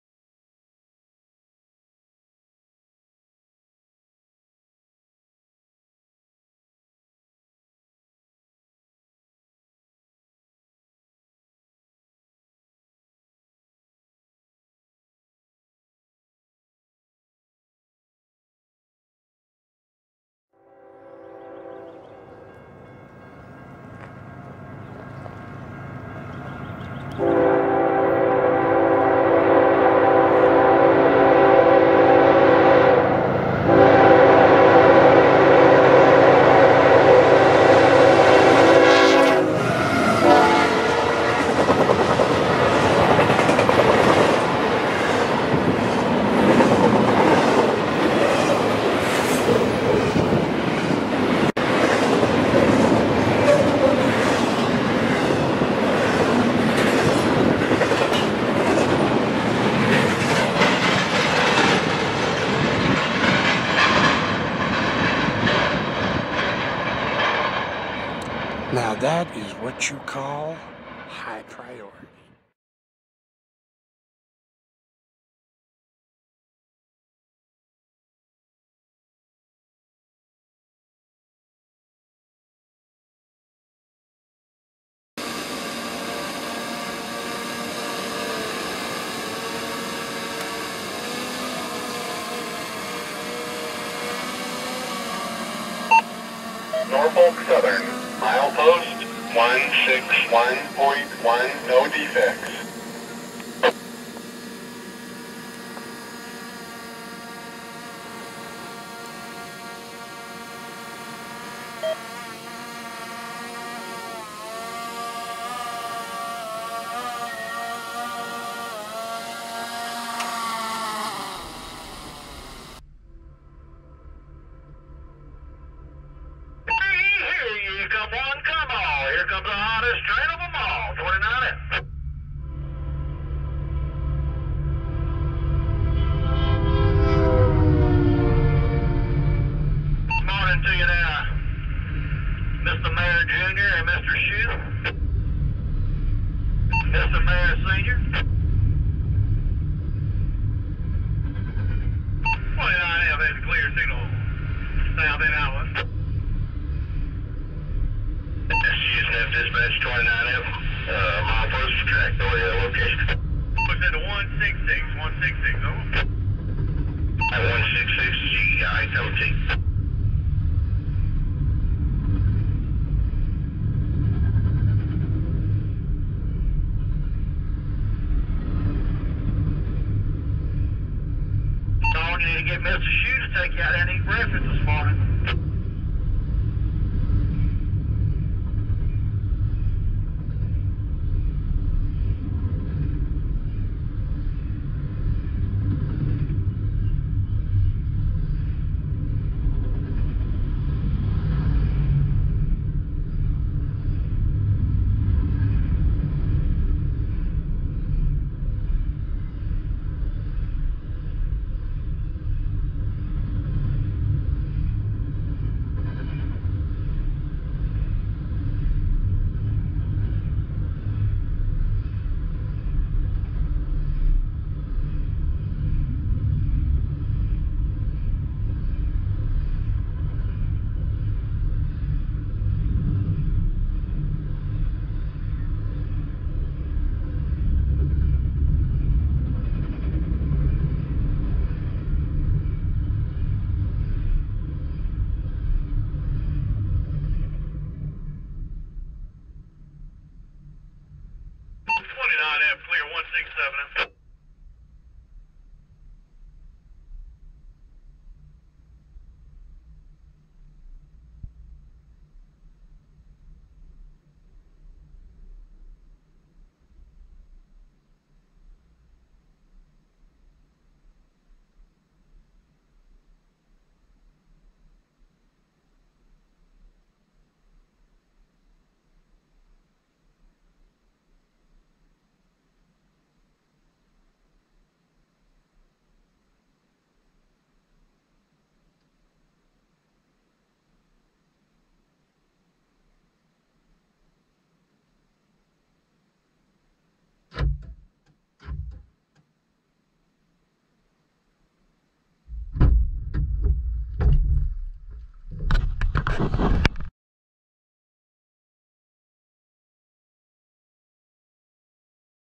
(🚂🍑)-[HQ]-S4E3-FOREIGN-POWER: UP's-In-The-Clear-Cold-Blue-Day-©-2023.wmv-(🍑🚂)
(💎) – |"HELLO-MY-FELLOW-RAILFAN-FRIEND'S"|
Here we are set up at The Milepost 165G of Howell on a Nice Clear and COLD Blue Day awaiting the arrival of NS 29-F with UP Power. Listen Close on the radio as Mr Sellars Mentions About NS 29-F being The Hottest of Them All.